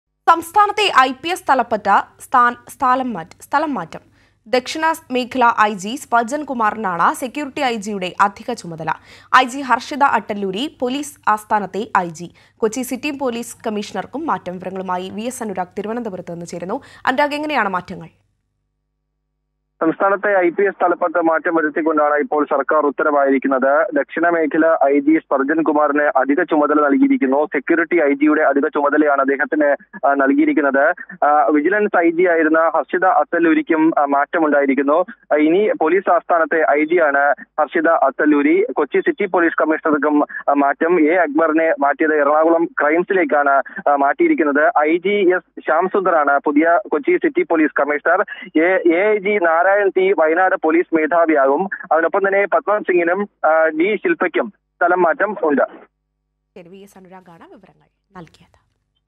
സംസ്ഥാനത്ത് ഐ.പി.എസ് തലപ്പത്ത് സ്ഥലംമാറ്റം | ips
സംസ്ഥാനത്ത് ഐ.പി.എസ് തലപ്പത്ത് സ്ഥലംമാറ്റം..ദക്ഷിണ മേഖല ഐ.ജി സ്പർജൻ കുമാറിന് സെക്യൂരിറ്റി ഐജിയുടെ അധിക ചുമതല നൽകി
Transfer of IPS head in the state... South Zone IG Spurgeon Kumar given additional charge of Security IG